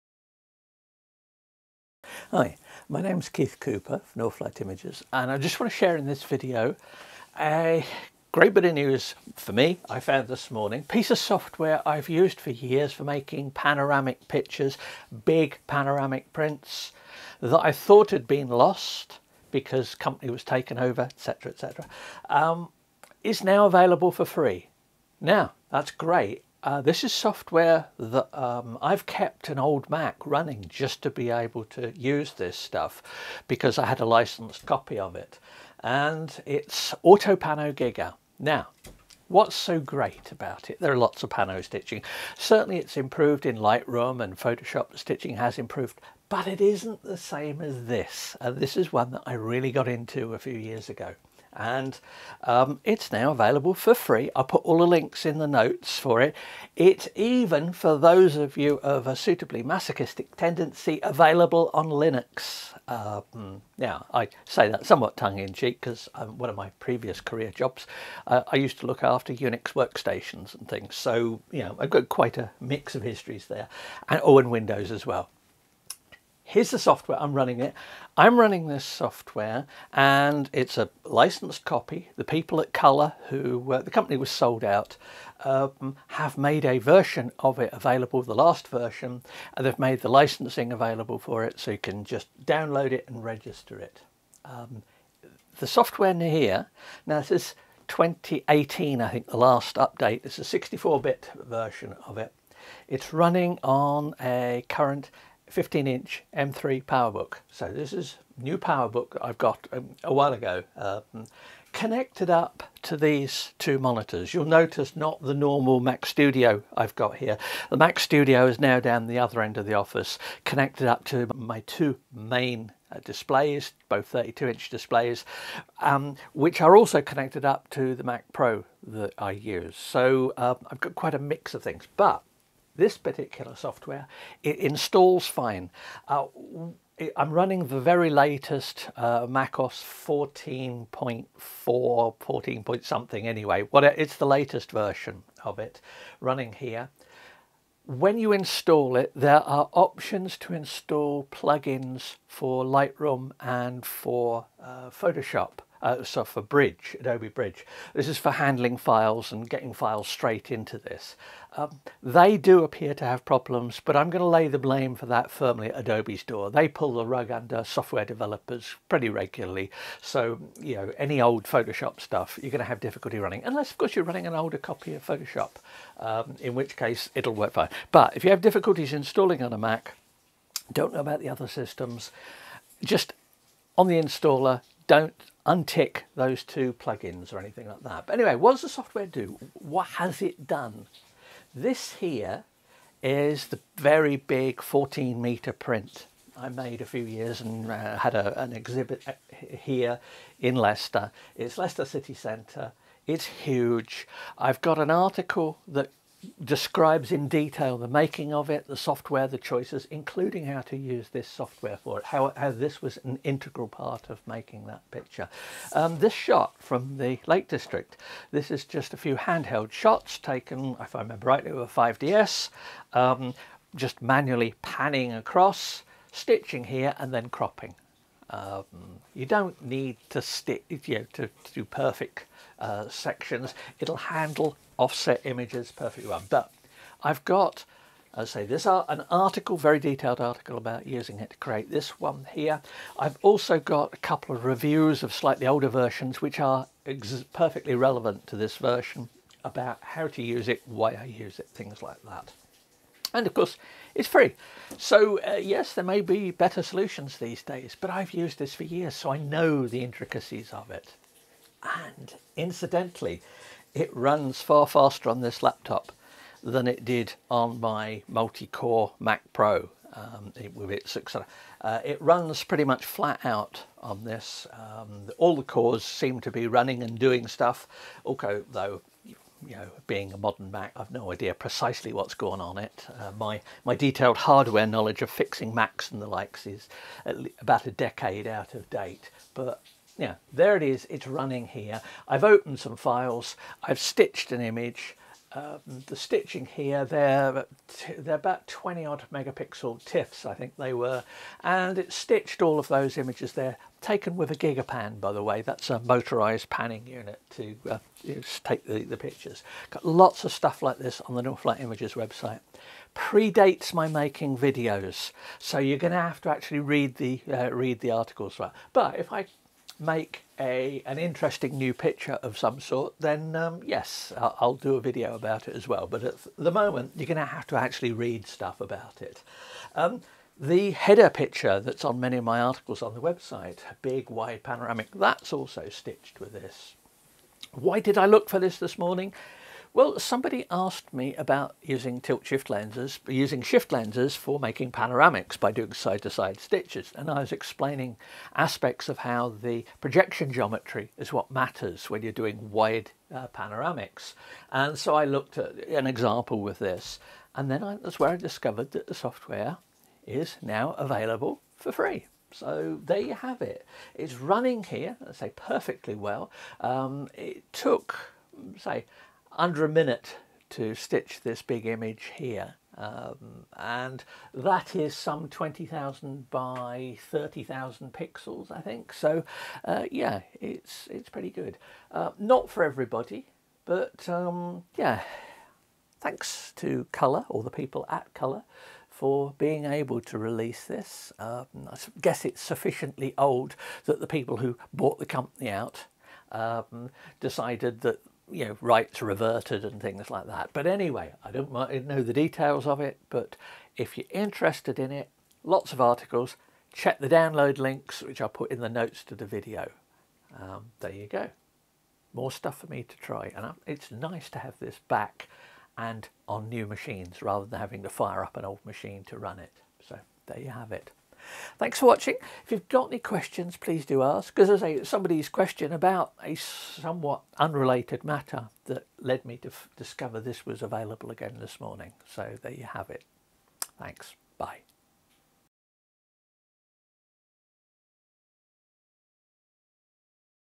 My favourite photo stitching software for free. I use Autopano Giga for my giant panoramic prints
I've just discovered that Autopano Giga pano stitching software is now available for free, for Mac, Windows and Linux. It's been my favourite choice for years.

It's the image stitching software I've used for many huge panoramic landscape and cityscape prints.

IMPORTANT: Read the notes on that page to avoid install difficulties
Don't forget the registration email and code just below the download links...

Some 'zoomable' high res images I've created in the past

An "APG for a big print" tutorial I've produced.

Using APG to create a 14 metre  long cityscape print

The developer web site now redirects elsewhere, but explore it in the Wayback Machine [archive.org]

My articles and videos are always free to access.
Any help with running this channel is gratefully received.
US Amazon photo/print gear
B&H Photo